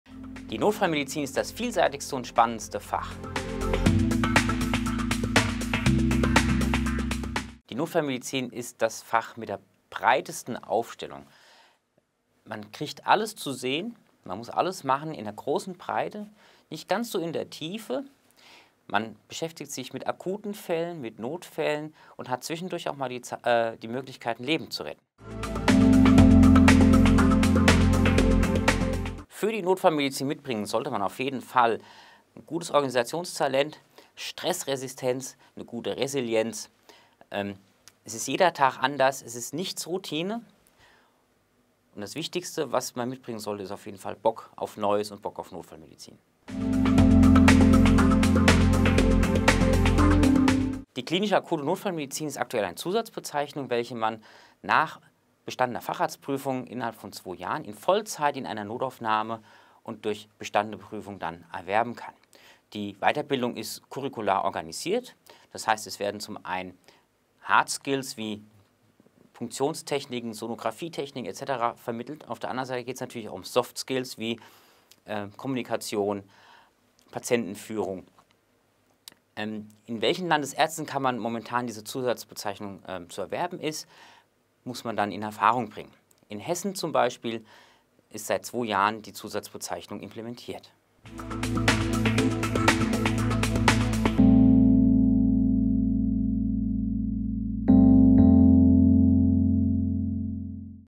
Nachwuchs Notfallmedizin: 3 Fragen 3 Antworten | Asklepios
In der Zentralen Notaufnahme ist kein Tag wie der andere. Akute Erkrankungen, Notfälle und das Retten von Leben stehen für die Mitarbeiter auf der Tagesordnung. Wer als Arzt in einer ZNA arbeiten möchte, muss nach dem Facharzt die Zusatzbezeichnung für klinisch akute Notfallmedizin erwerben. Patrick Müller-Nolte, Ärztlicher Leiter der Zentralen Notaufnahme an der Asklepios Stadtklinik Bad Wildungen
erklärt, was die Notfallmedizin auszeichnet, worauf es für Bewerber ankommt und wie die Weiterbildung organisiert ist.